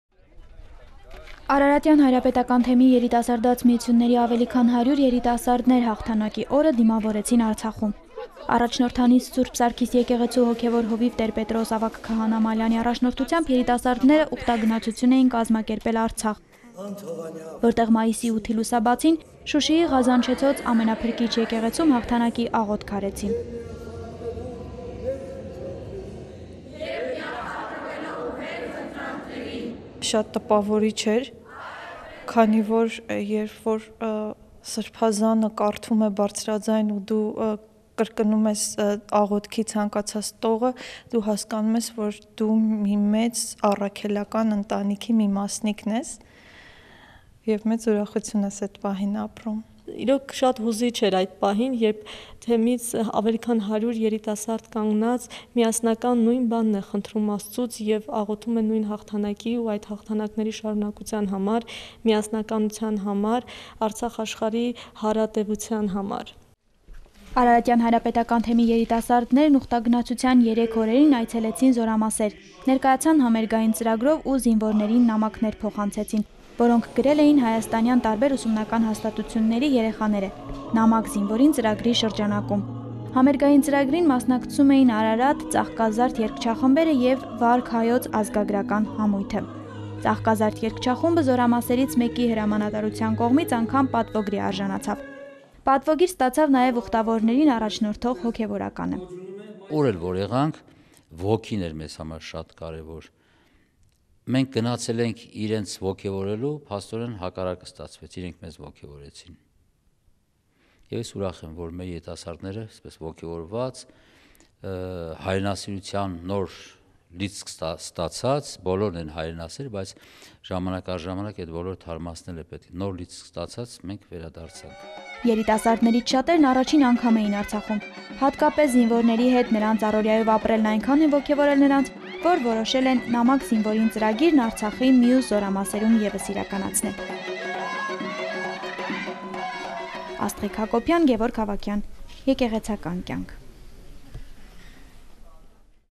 Pilgrimage to Artsakh
Over 100 youth members of the Araratian Pontifical Diocese made a pilgrimage to Artsakh, led by Rev. Fr. Petros Malyan. During their pilgrimage they visited army units and gave performances.

Ուխտագնացություն Արցախ

Ավելի քան 100 երիտասարդներ Արարատյան հայրապետական թեմից Տ. Պետրոս ավագ քահանա Մալյանի առաջնորդությամբ ուխտագնացություն էին կազմակերպել Արցախ: Այս ընթացքում նրանք այցելեցին զորամասեր, ներկայացան համերգային ծրագրով: